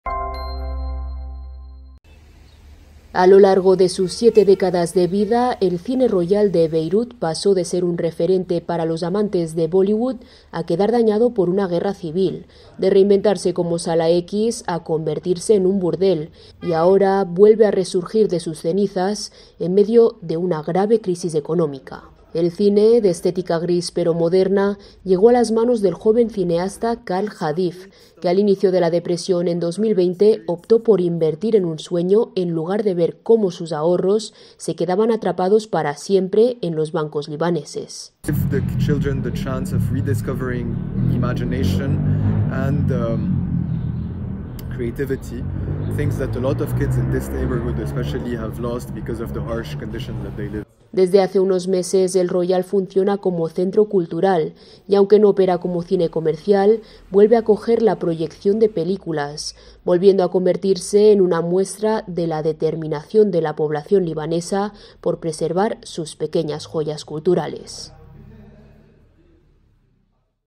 Un cine beirutí paseó por la gloria y la ruina... pasando por una sala X
Beirut (Líbano), 19 abr (EFE).- (Imagen: Ana María Guzelian) A lo largo de sus siete décadas de vida, el beirutí Cine Royal pasó de ser un referente para los amantes de Bollywood a quedar dañado por una guerra civil, de reinvertarse como sala X a convertirse en un burdel y, ahora, vuelve a resurgir de sus cenizas en medio de una grave crisis económica.